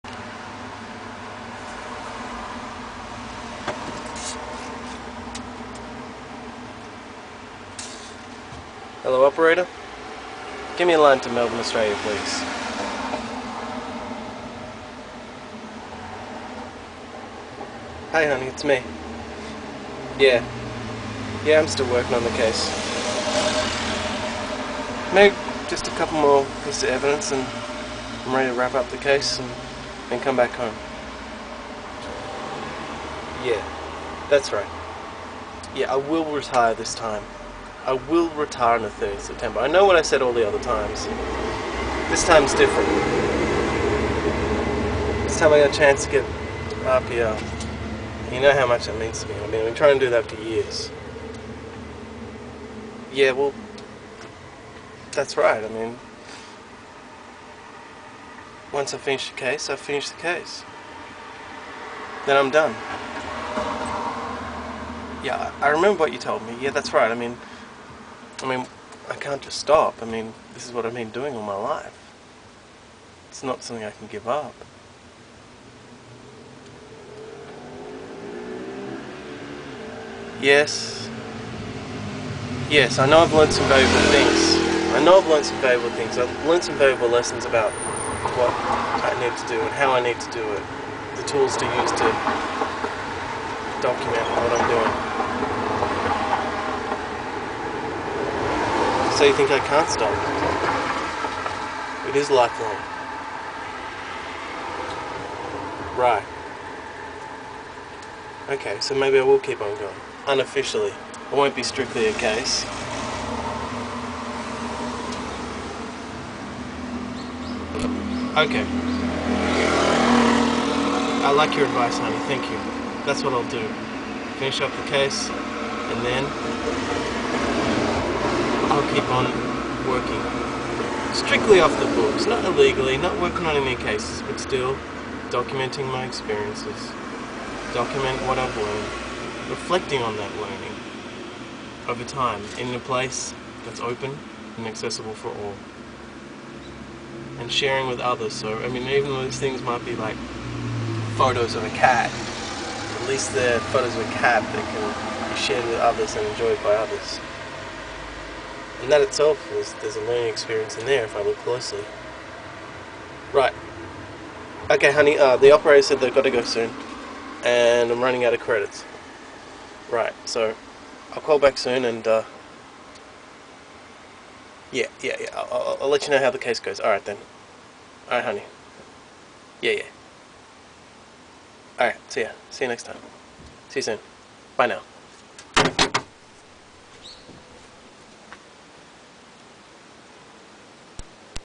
Detective Peterovski calls home to discuss his life after retirement
A homesick Detective Peterovki calls home to talk to his wife about the EpCoP Case and what he can do with himself once he's retired. Detective Peterovski has a hunch, there's always something to be investigated. Once you've closed a case, it doesn't always remain closed or another case will open.

Detective Ryan Peterovski is a detective who's retiring on 30th September 2011. Has one last case to solve.

Find out more about Detective Peterovski and his EpCoP MOOC Case

The E-portfolio Communities of Practice Massive Open Online Course or EpCoP MOOC ran from 1 August - 30 September 2011. The aim of the EpCoP MOOC was for participants to learn tools and technique for creating and reflecting in their own e-portfolio or online space.